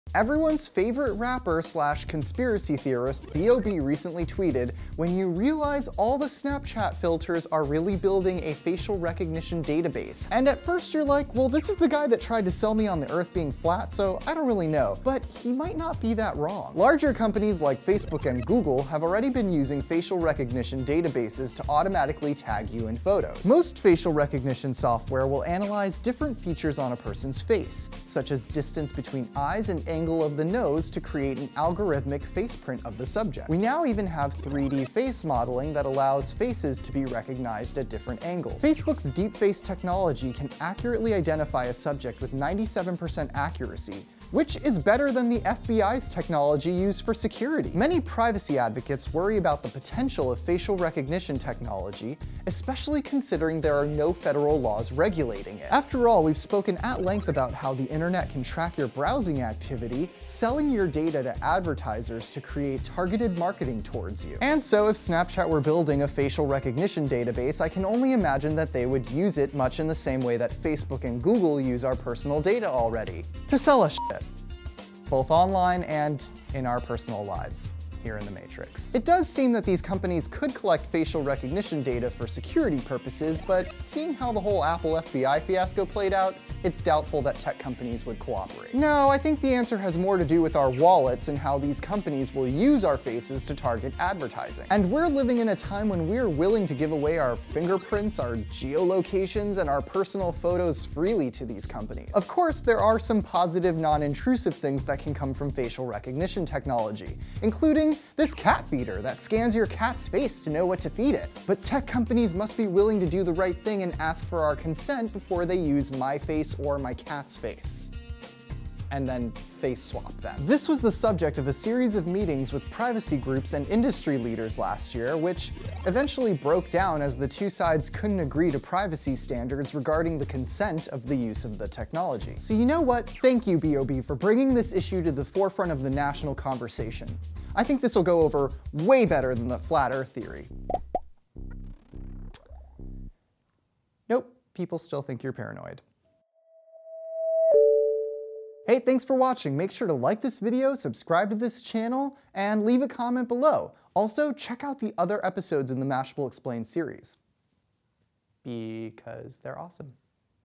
How Facebook Could Steal Your Face | Mashable Explains
Facial recognition technology is becoming more and more prevalent in the technology we use today - from Facebook tagging to Snapchat lens filters.

But is it possible that these companies are using this data for other purposes, and without our consent?

Hosted by Armand Valdes
@ArmandTweets on Twitter

Sources:

Defeat for Facebook in Court Is Bad News for Firms That Scan Faces

Is facial recognition a threat on Facebook and Google?

DeepFace: Closing the Gap to Human-Level Performance in Face Verification

Facial Recognition Market Expected to Reach US$ 2.67 Bn by 2022 Globally